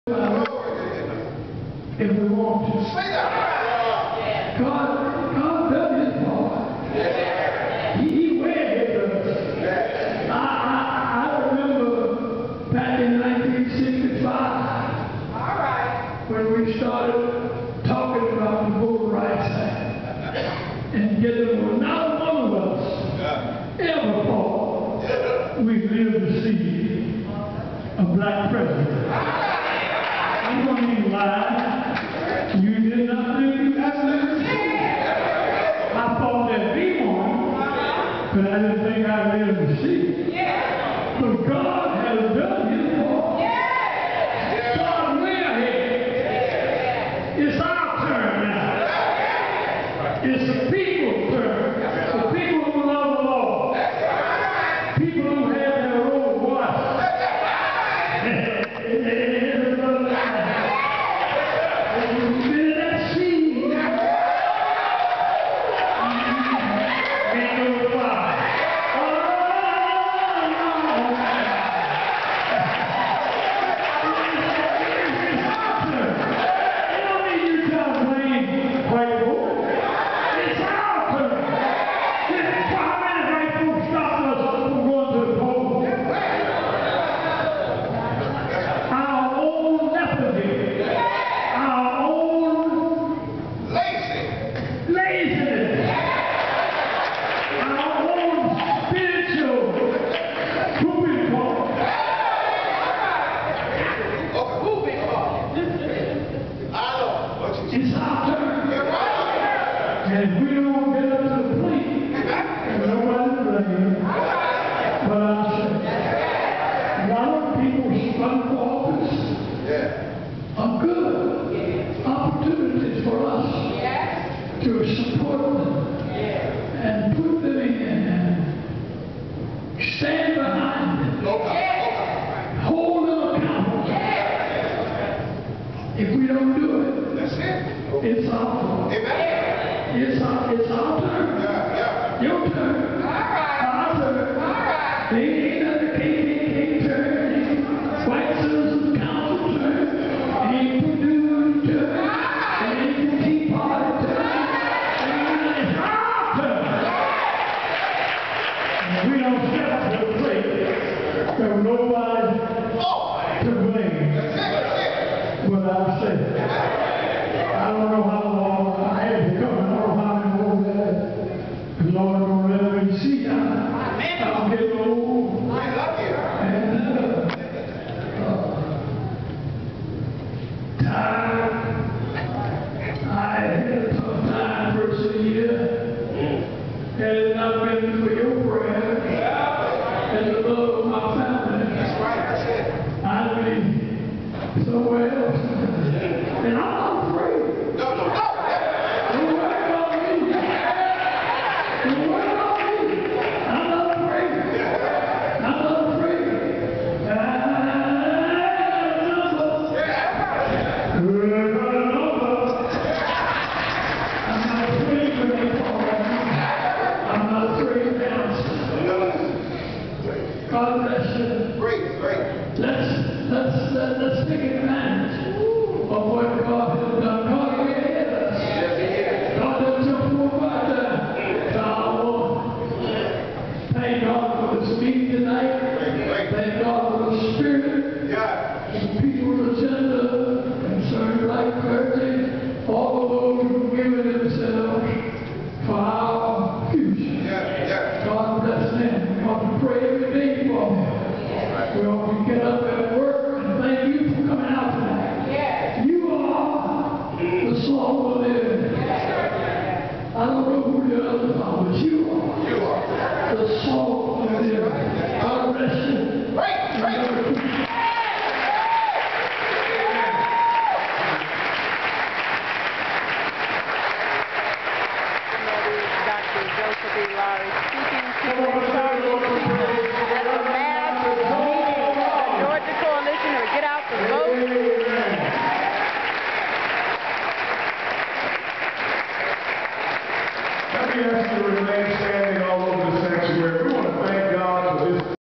Dr. Joseph Lowery at GOTV Mass Meeting Call To Action for Election 2010.AVI
Georgia Coalition to Get Out The Vote Mass Meeting on the eve of the 45th Anniversary of The Voting Rights Act  with Civil Rights Icon  Dr. Joseph E. Lowery
A Coalition of clergy, ministries, women, youth and other organizations.come together for the Statewide GOTV Campaign.  Denominations represented with civil and civic advocate and activists for the Primary Runoff Election for Tuesday, August 10, as clergy is challenged for August 8 to bring the message for Civic Sunday.